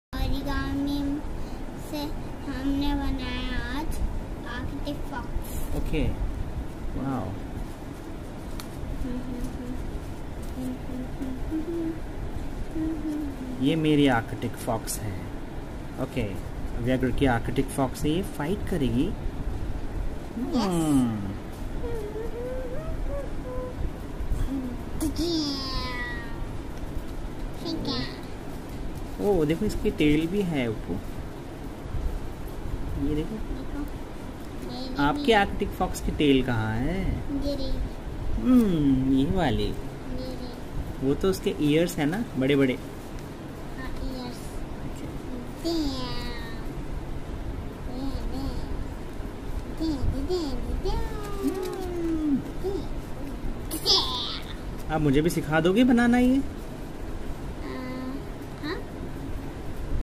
Origami Arctic Fox🦊
Today we made Arctic Fox.Origami Art is a paper Art. We can make paper art  and tell our children to do the activity.
Origami is a paper art of Japanese. Includes the folding of paper.